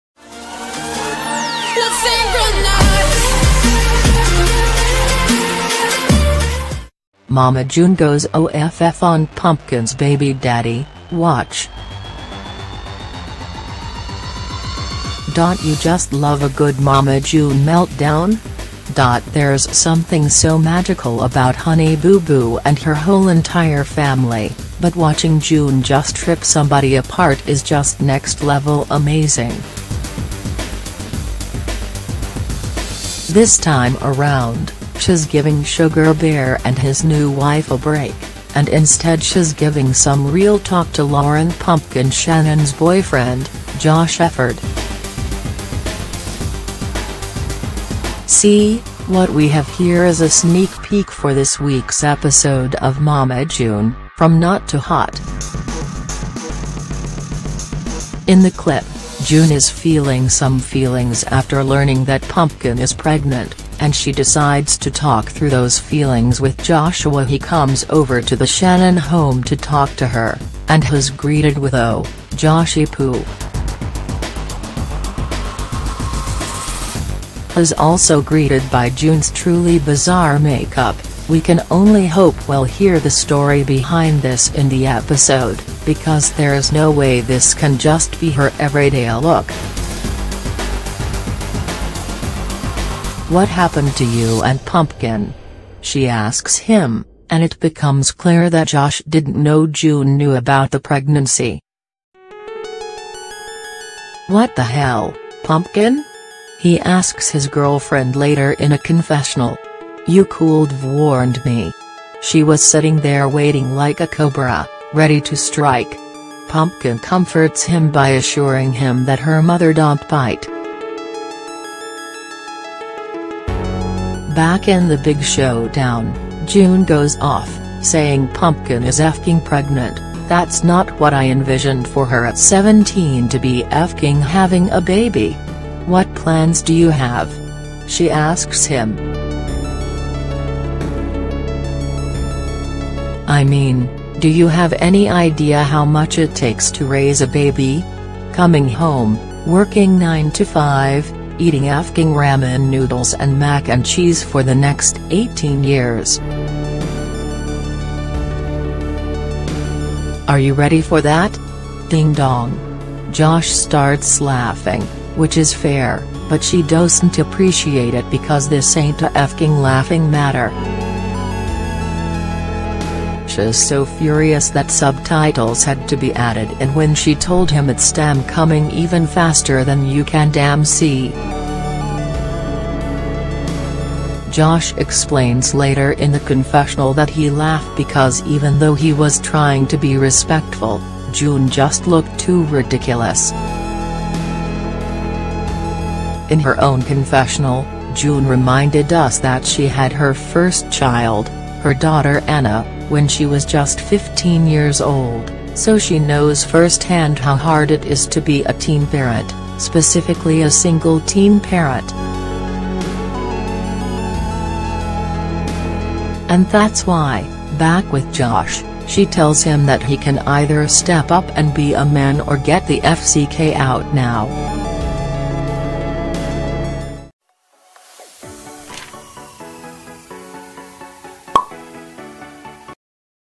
Mama June Goes OFF on Pumpkin's Baby Daddy: Watch! | 24H News
Mama June Goes OFF on Pumpkin's Baby Daddy: Watch!
Don't you just love a good Mama June meltdown?!